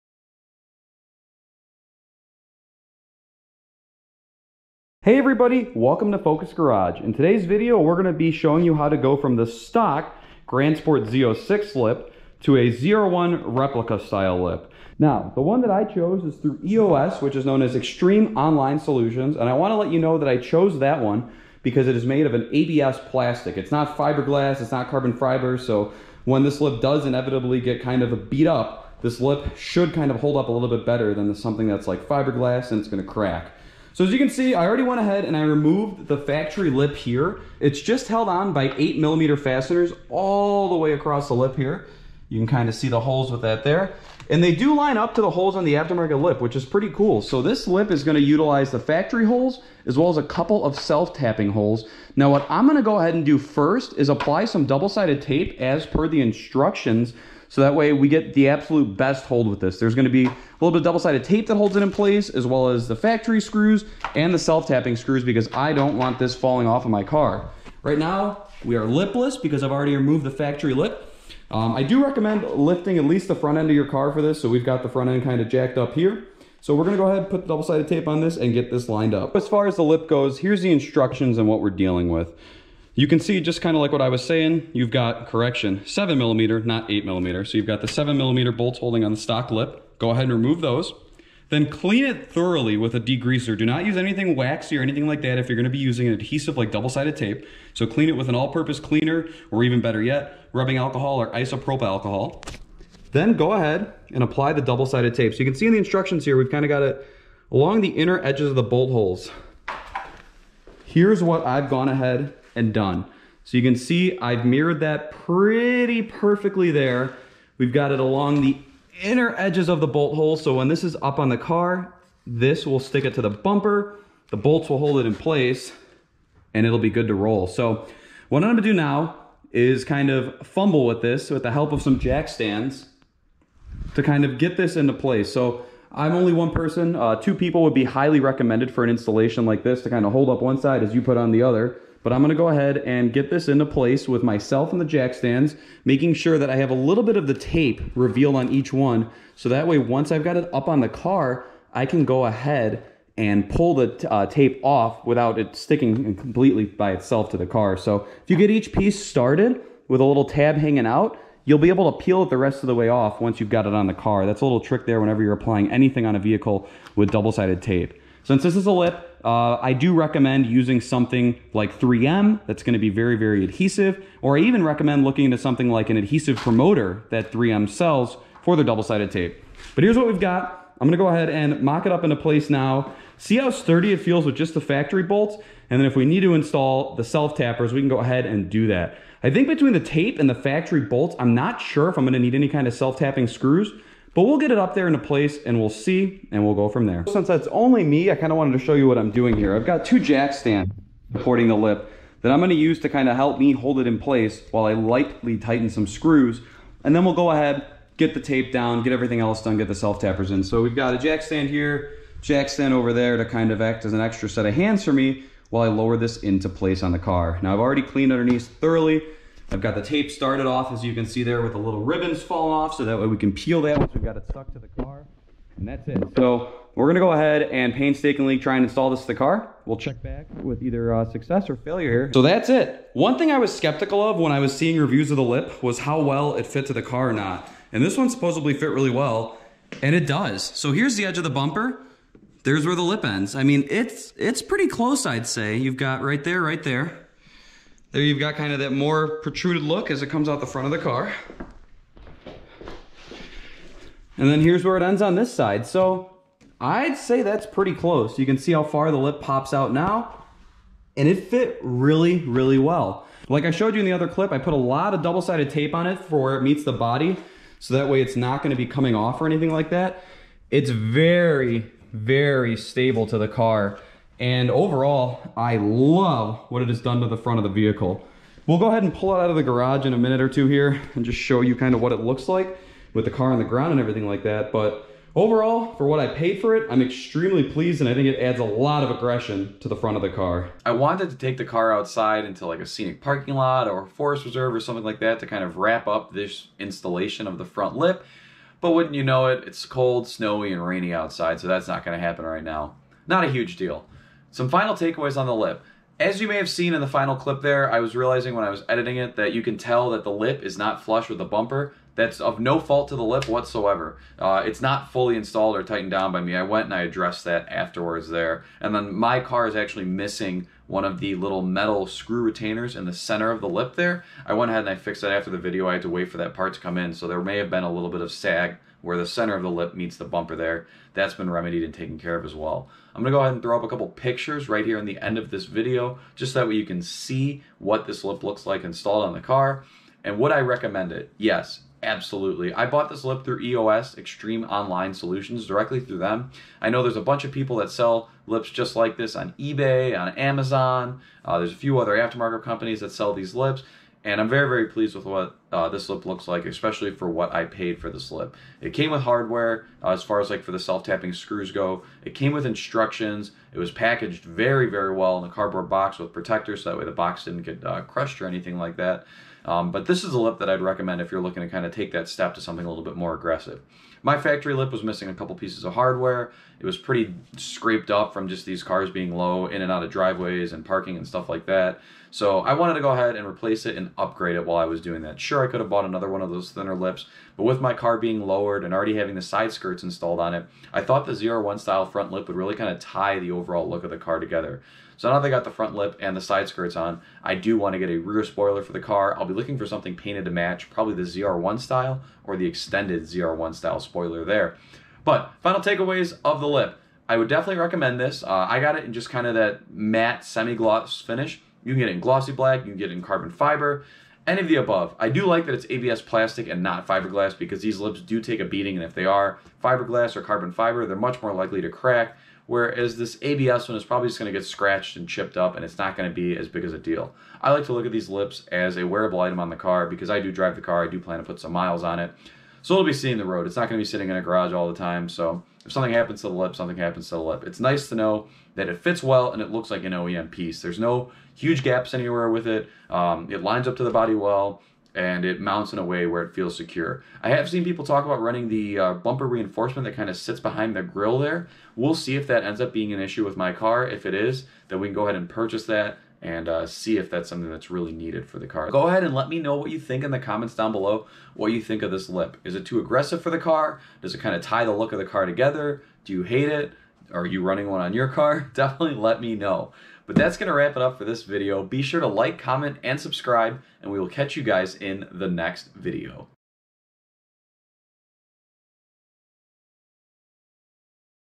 ZR1 Style Front Lip Installation | C6 Widebody Corvette
Time to replace the worn front lip on the Grand Sport with something much more aggressive looking! This video shows you how to install the EOS ZR1-style front lip on a widebody C6 Corvette.